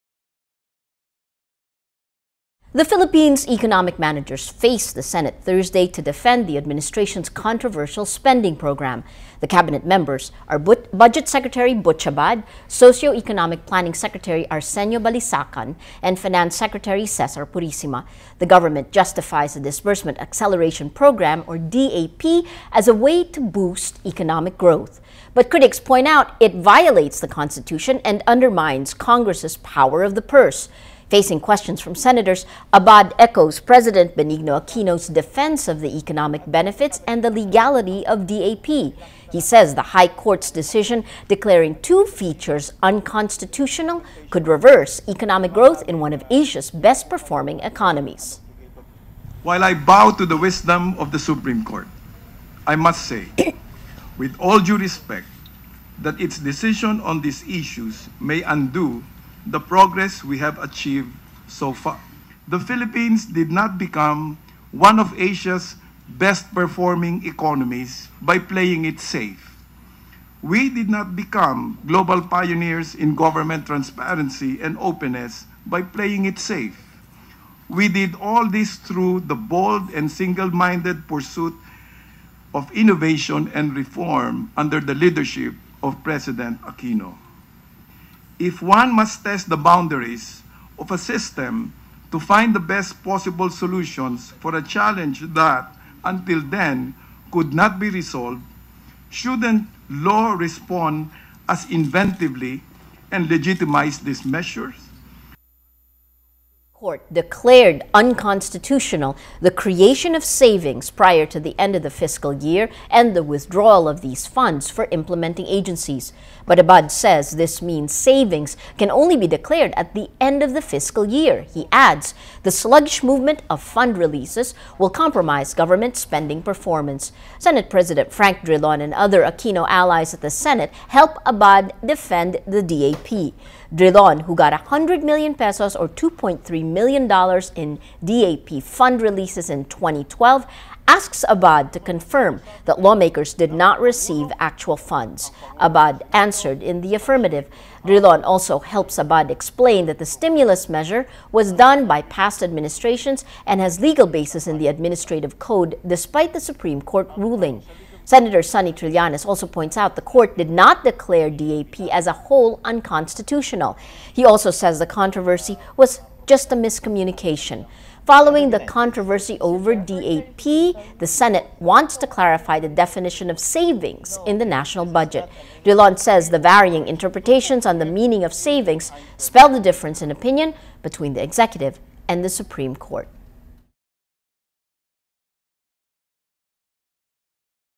Abad: SC ruling may undo economic progress
More videos on Rappler Follow Rappler for the latest news in the Philippines and across the globe. FULL STORY

The Philippines' economic managers face the Senate Thursday to defend the administration's controversial spending program. Follow Rappler for the latest news in the Philippines and around the world.